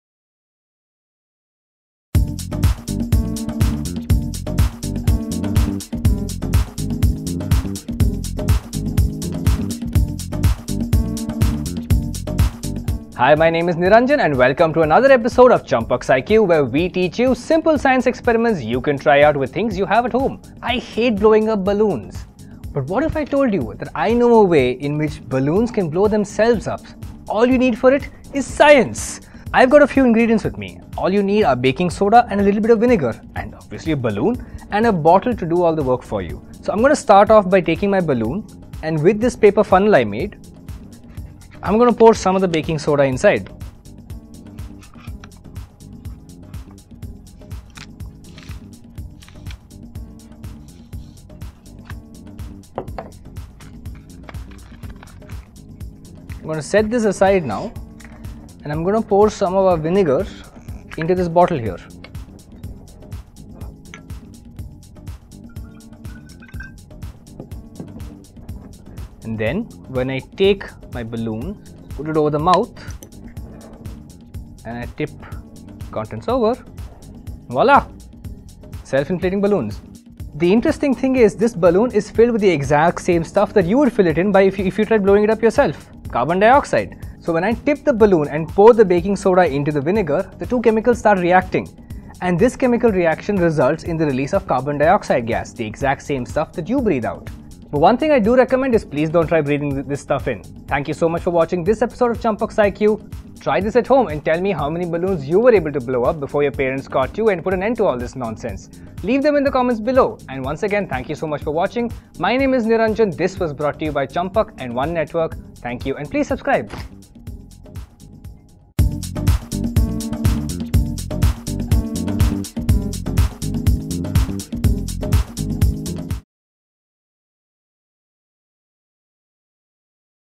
Self Inflating A Ballon - Kids Science Experiments
In this simple and fun experiment, we will teach you to blow a ballon, but the tiwst is they will blow up themselves. Confused ??
Just watch the video, you will get your answer.

Here are all the things you'd need:

1) A Balloon
2) Baking Soda
3) Vinegar
4) A Empty Bottle